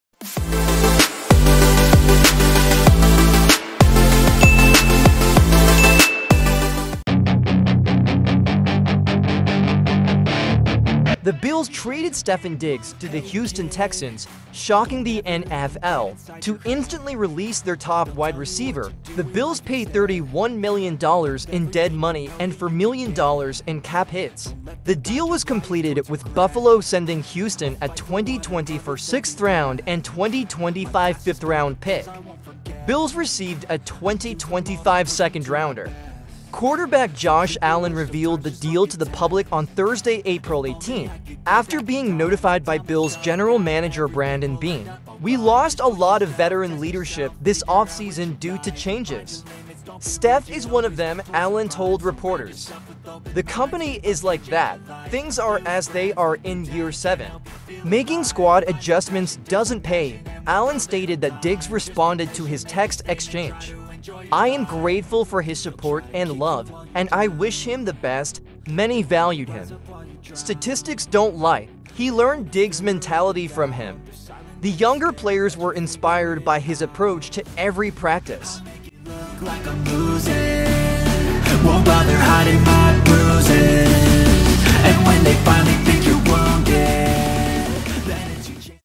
Bills QB Josh Allen’s Text to Stefon Diggs After Surprise Trade
The Bills traded Stefon Diggs to the Houston Texans, shocking the NFL. To instantly release their top wide receiver, the Bills paid $31 million in dead money and $4 million in cap hits.